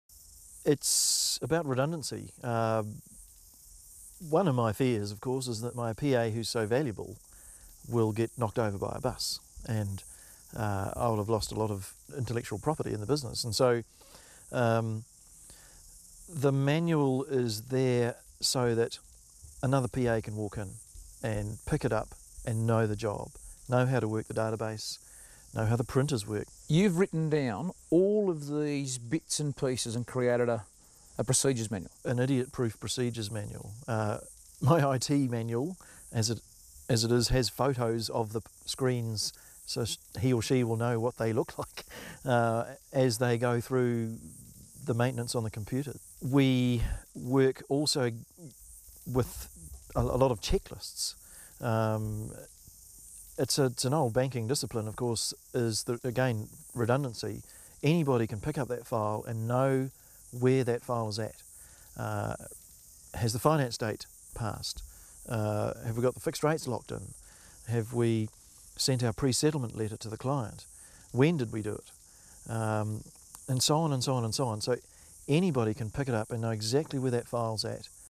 Mortgage Brokers - Procedure Manuals
Darrell Weekes interviews Finlay Abbot